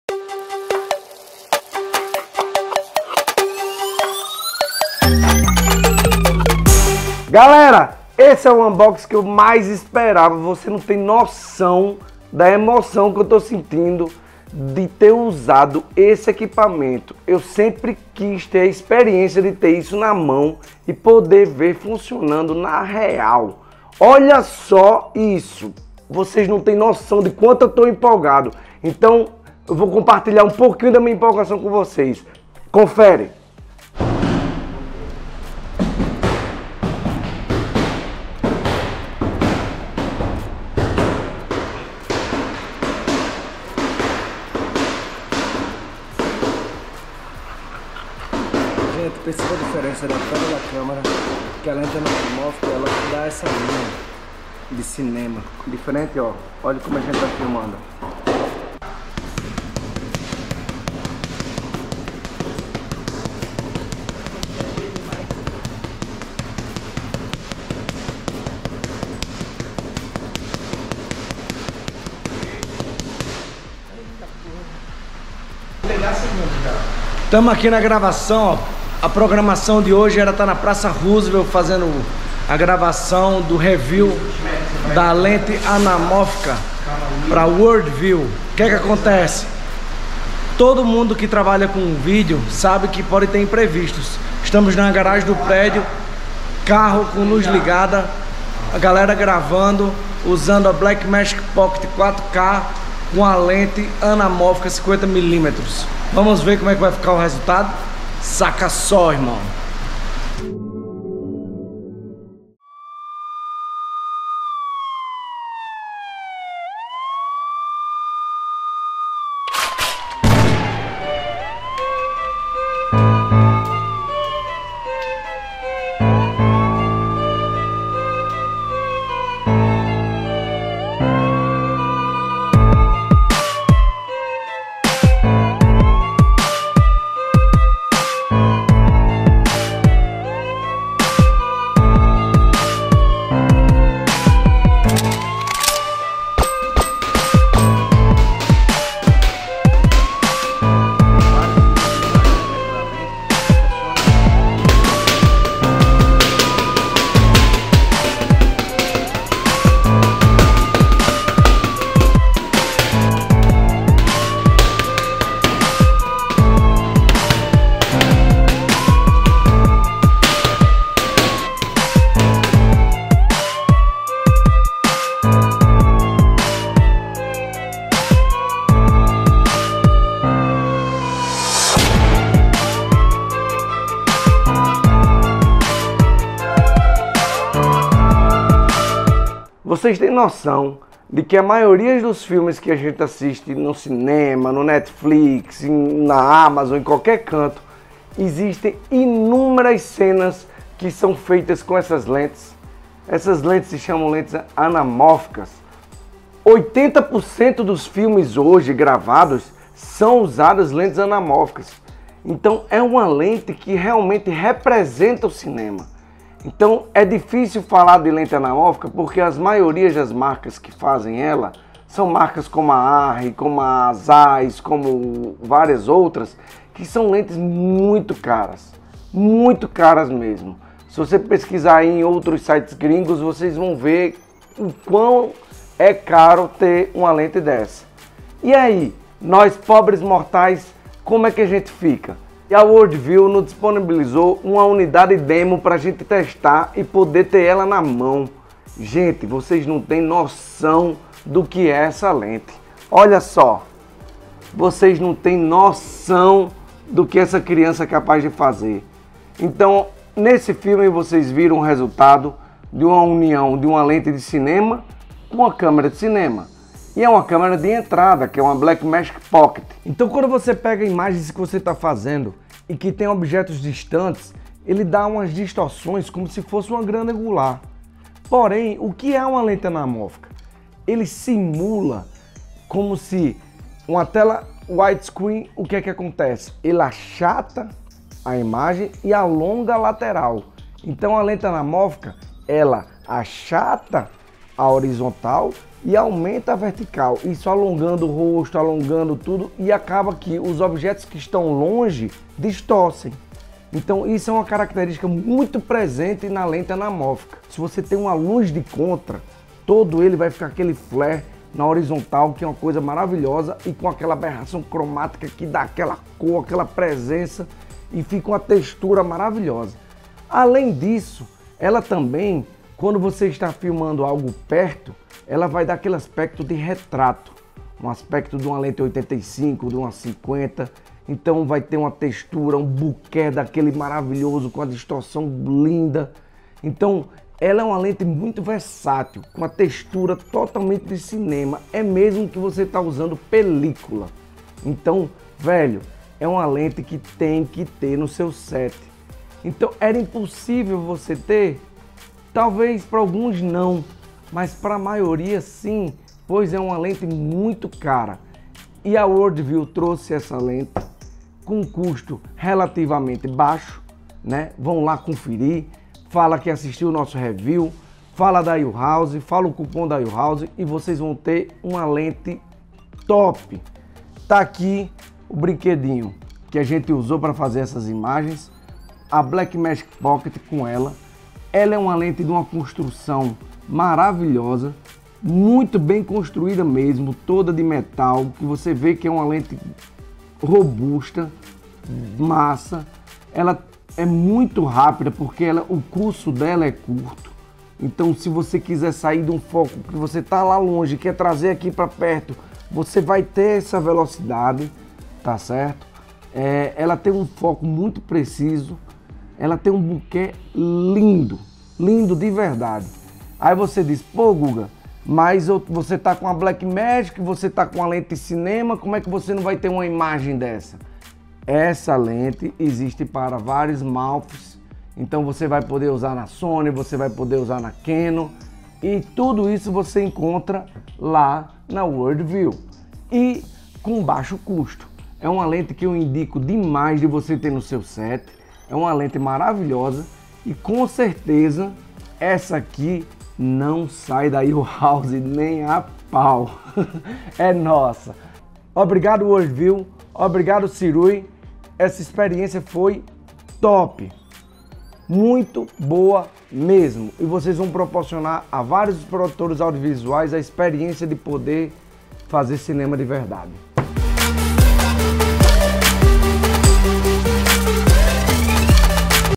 Review Completo LENTES Anamórficas SIRUI 50mm F1.8 Anamorphic REPRESENTATIVIDADE DO CINEMA
Efeito Bokeh Oval
🌎Crie sua própria arte cinematográfica com um simples giro do anel de abertura da Lente Anamórfica Sirui 50mm f/1.8 para otimizar a profundidade de visão e o efeito bokeh.

Curtiu? Adquira o seu pelo nosso site!

 Um aspecto fundamental das Lentes Anamórficas "Anamorphic" é o bokeh oval destacado, e essa Lente 50mm Anamórfica atinge uma profundidade de campo rasa e o bokeh oval com suas 10 lâminas de abertura. Embora a Lente tenha um Corpo Robusto, ela também foi projetada para ser leve para uso com sua Câmera Mirrorless Compacta Micro Four Thirds de Formato APS-C.